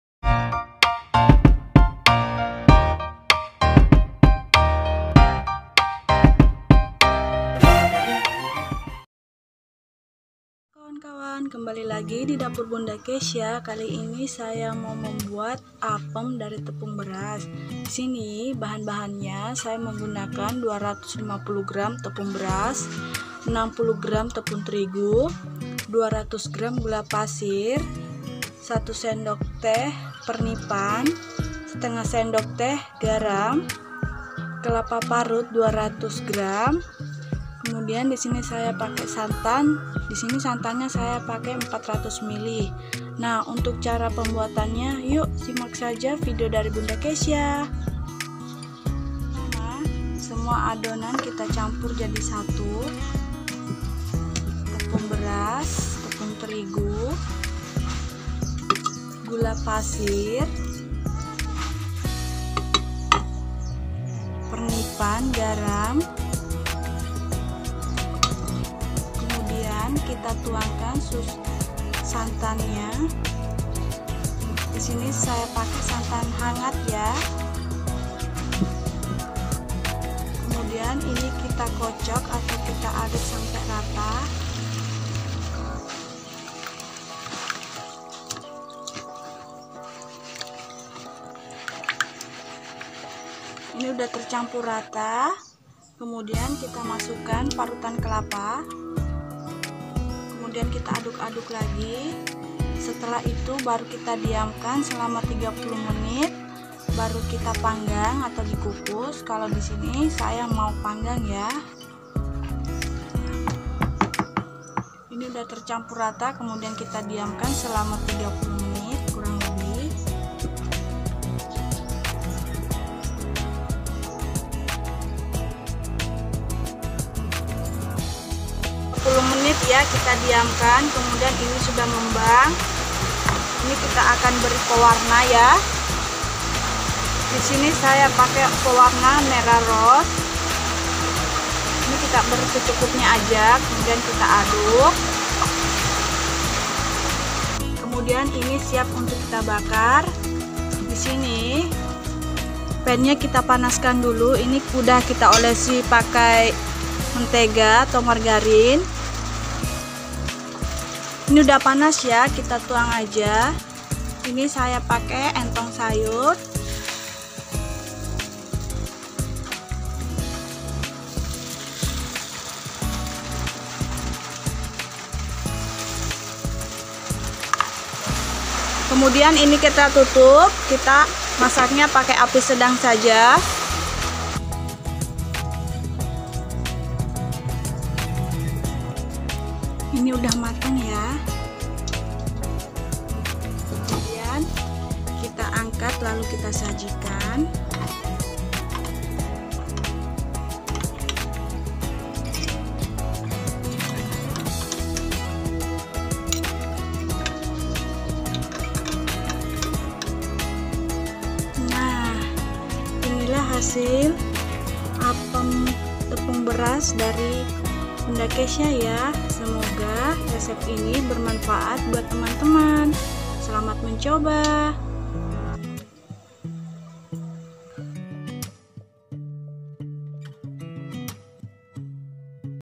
RESEP CARA MEMBUAT APEM TEPUNG BERAS
Assalamualaikum Sahabat Bunda Keysha

Kali ini saya mau berbagi resep cara membuat apem yg menul menul terbuat dari tepung beras,..
Nah buat teman teman yang mau tahu resep & cara pembuatan yuk simak saja videonya sampai selesai.

Semoga video ini bermanfaat buat teman teman ya..

Terima kasih
Happy cooking
Selamat Mencoba